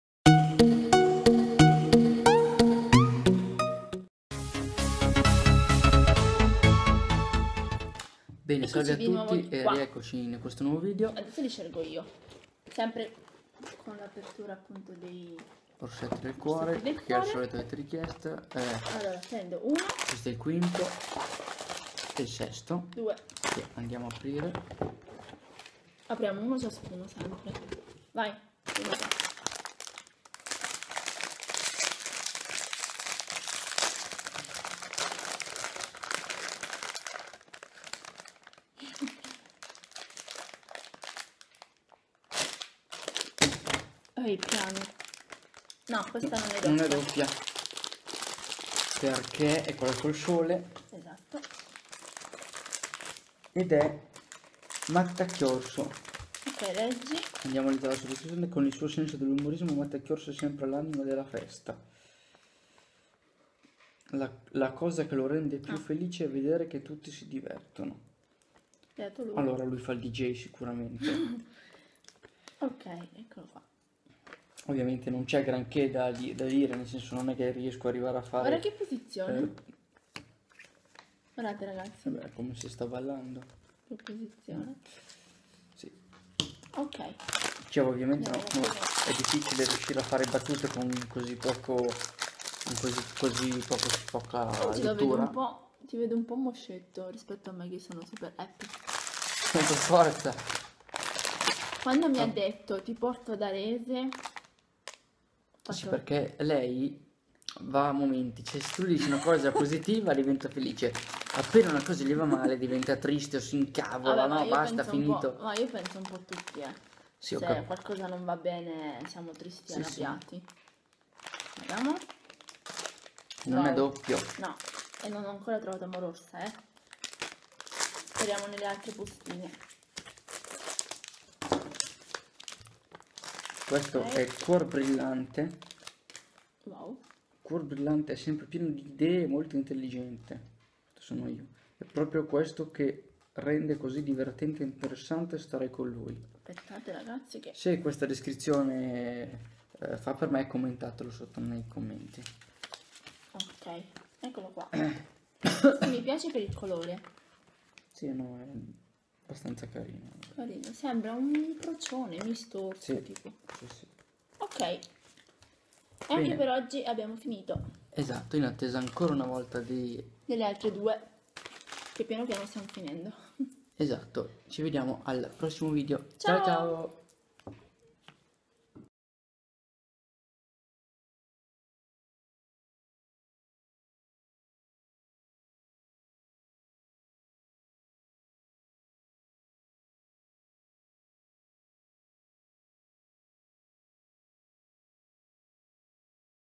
#4 APERTURA ORSETTI DEL CUORE
In questo video vi mostreremo un'altra apertura degli orsetti del cuore. Buona visione a tutti/e.

Commentate il video per dare una vostra opinione e cliccate sul pollice in su se vi è piaciuto.

INSTAGRAM: Chiara Kawaii and Marco Cerry